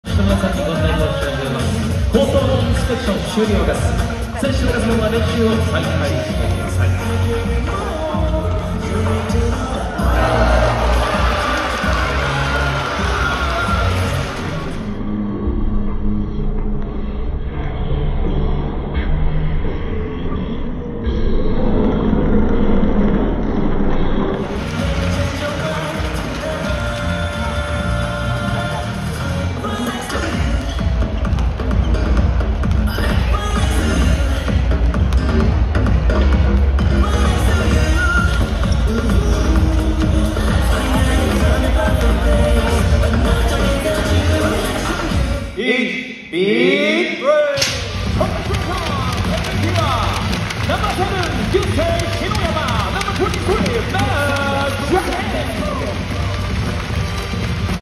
【ベンチからのスーパーショット】マットジャニング選手（川崎ブレイブサンダース）
川崎ブレイブサンダースの練習前には選手達がベンチからのショットを試します。

この日はマットジャニング選手がチャレンジし、見事入りました。

スーパーショットが入った後に行われた「21-22 B.LEAGUE CS準々決勝第2戦（vs 名古屋ダイヤモンドドルフィンズ）」のMVPに選出されました。

今後も試合前のとどろきアリーナのベンチから目が離せません。

お出かけの際の参考にしていただければ幸いです。

現地の様子はこちら。

【Profile】
出身校：ノースイースタン大学
出身地：アメリカ合衆国
生年月日：1988年6月22日
身長(cm)：196cm
体重(kg)：99kg
国籍：アメリカ合衆国
ニックネーム：マット